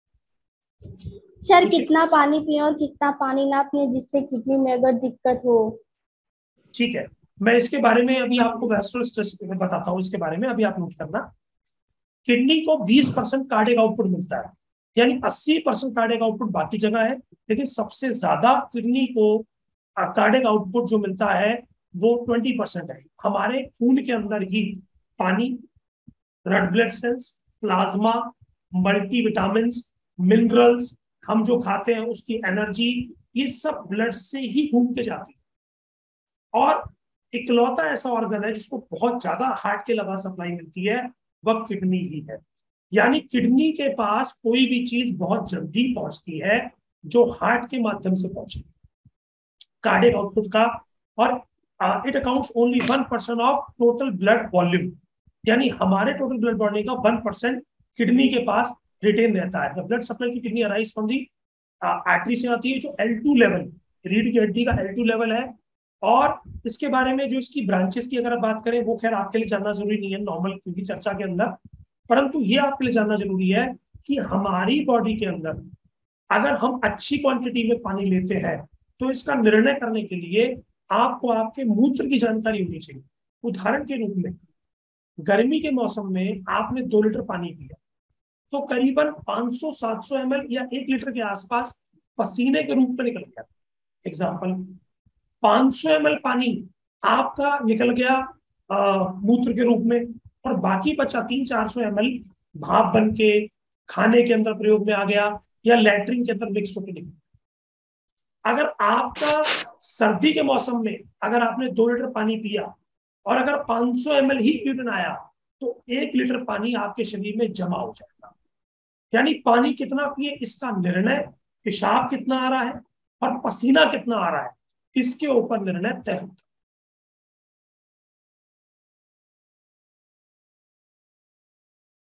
जानिय कितना पानी पीने से होगा हमारी किडनी को फायदा
अब आप भी घर बैठे जान सकते है किडनी से जुडी सभी समस्याओ का समाधान
इस Kidney Worrier Program के द्वारा |
और जान सकते है अपनी किडनी की समस्या के सवालों के जवाब इस Webinar में |
13 August 2022 - शनिवार सुबह 11 बजे |